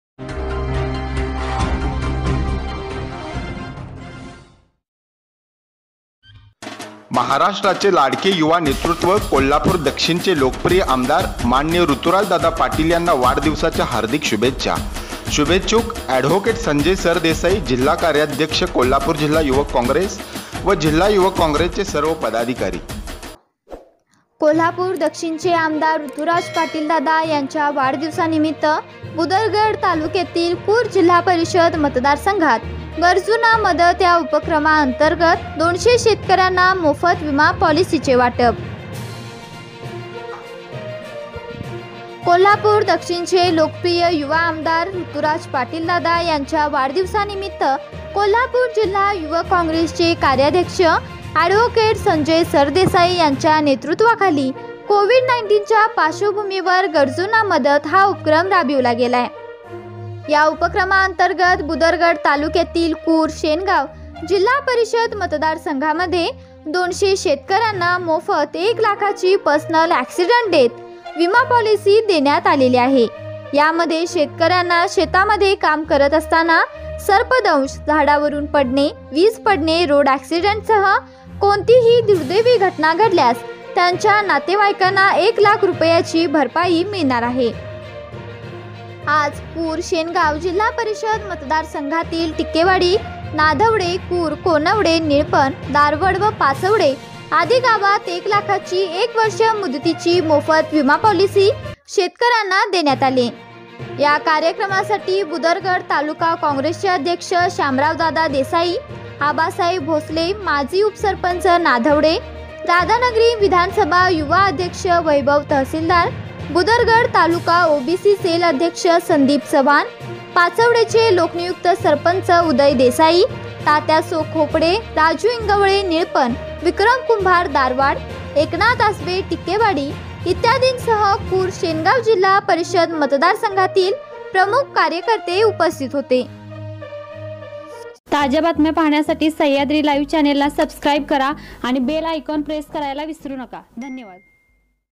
आम.ऋतुराज पाटील यांच्या वाढदिवसानिमित्त कुर मतदार संघात शेतकऱ्यांना विमा कवच. -ॲड संजय सरदेसाई
आपण पाहत आहेत सडेतोड आणि पारदर्शक सहयाद्री Live न्युज चॅनल.आपल्या परिसरातील घडणाऱ्या घडामोडी,घटना,समस्या,सामाजिक उपक्रम आदींच्या प्रसारणा बाबत तुम्ही आम्हाला संपर्क करू शकता.